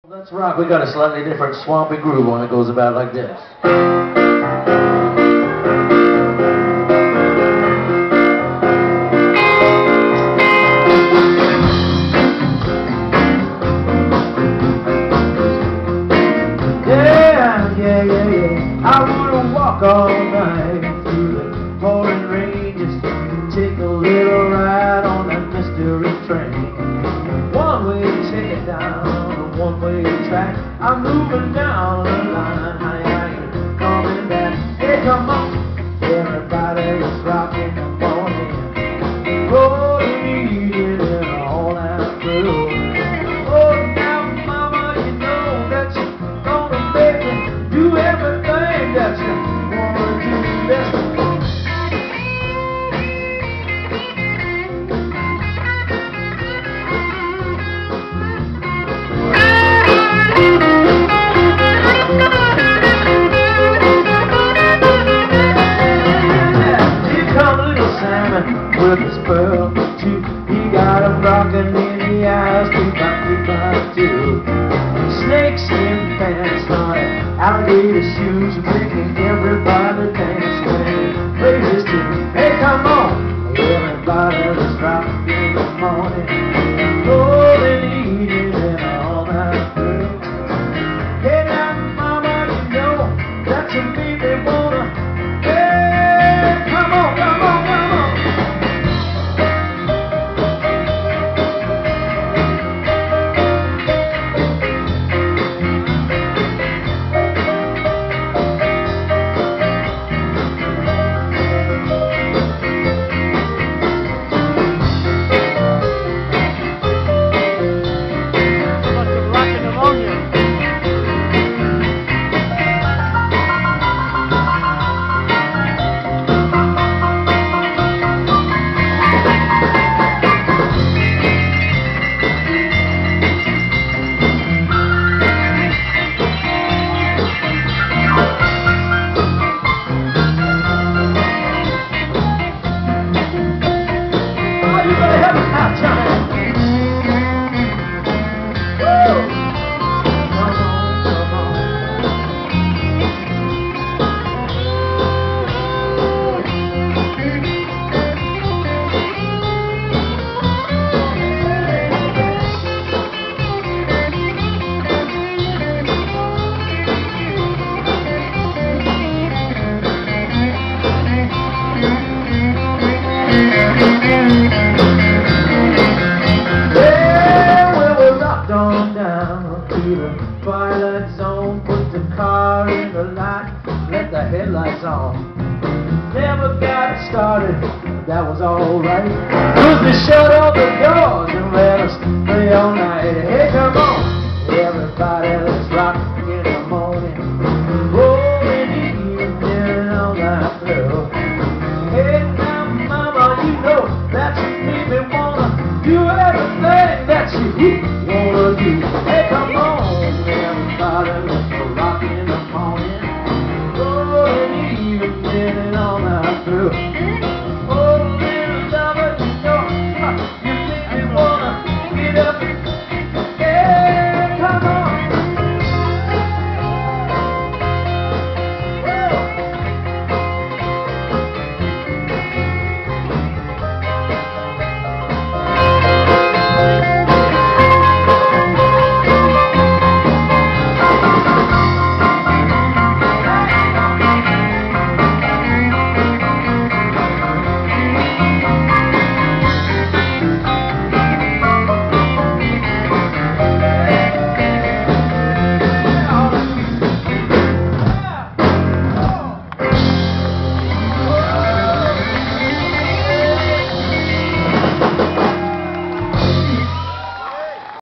Norton Buffalo's Knockouts
Shot this on 7/4/10  at the Water Front Blues Festival in Portland Oregon. Good band! Nice sound!